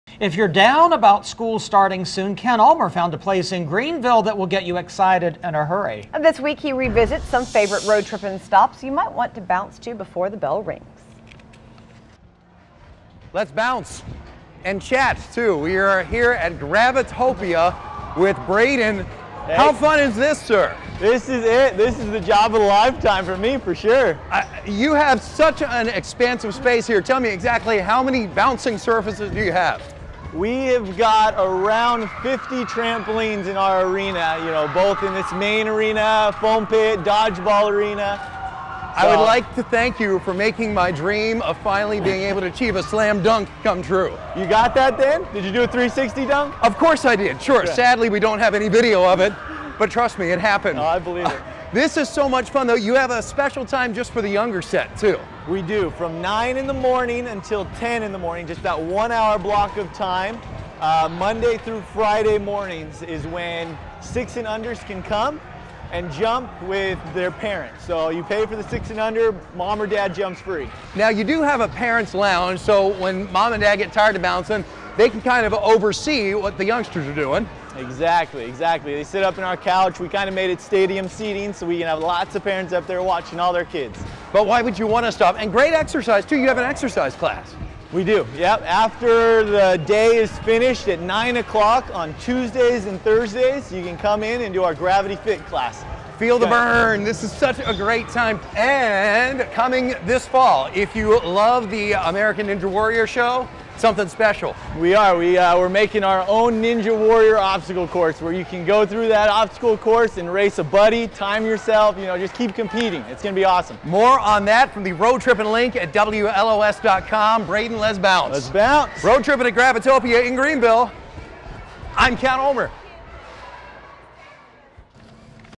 Ken's Favorites- Gravitopia
Get the family moving in this amazing trampoline arena!